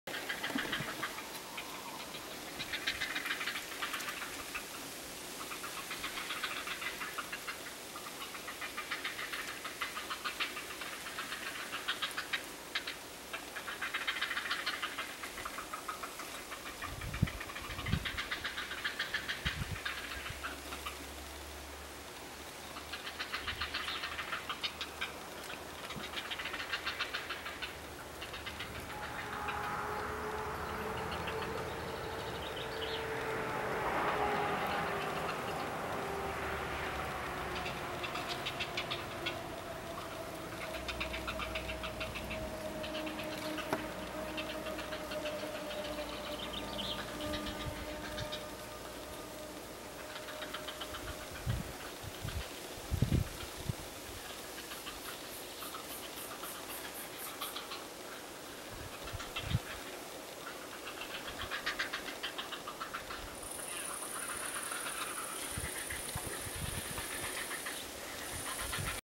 Crias de Garça Real
Garça Real a alimentar exigentes crias